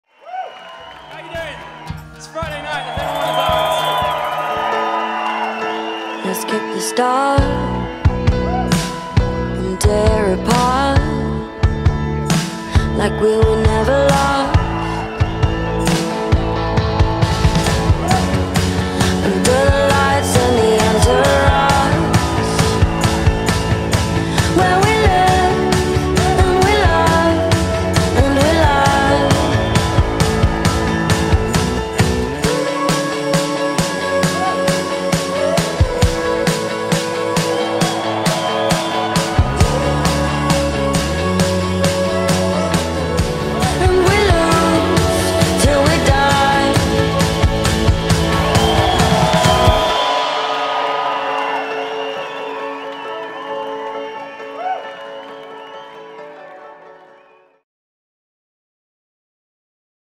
The Thatchers Haze Sessions 2016
A night of live music from some of the UK's most up and coming performers, hosted by BBC Radio 1 DJ, Huw Stephens.